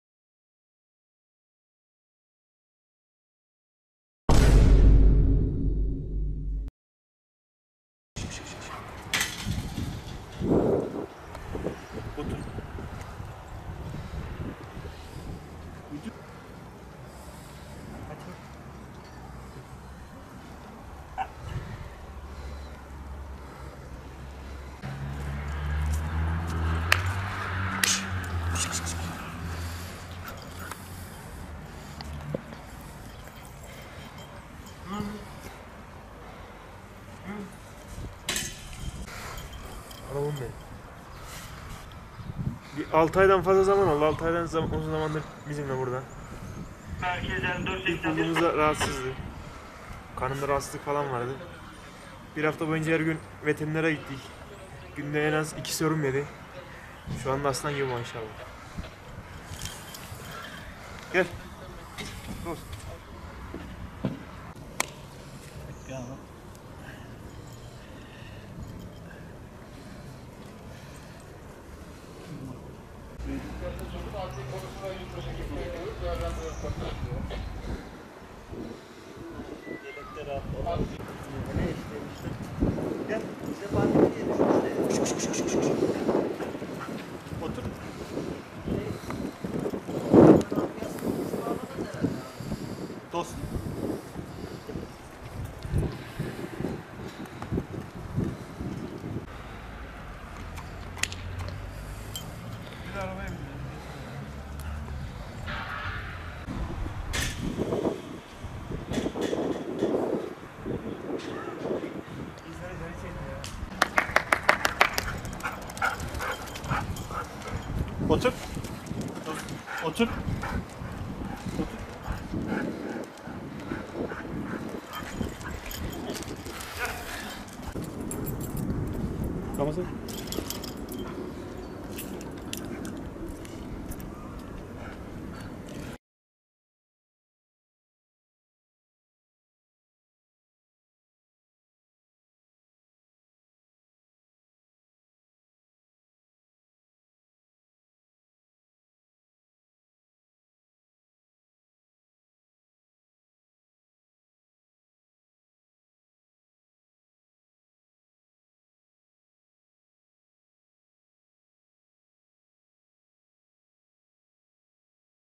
Polisler ölmek üzereyken buldukları köpeği sağlığına kavuşturdu
Bolu’da, polisler tarafından hasta ve ölmek üzereyken bulunan Sibirya Kurdu cinsi köpek, veteriner hekime götürülerek sağlığına kavuşturuldu. Sahibi bulunamayan ve ‘Dost’ ismi verilen köpek, emniyetin maskotu haline geldi.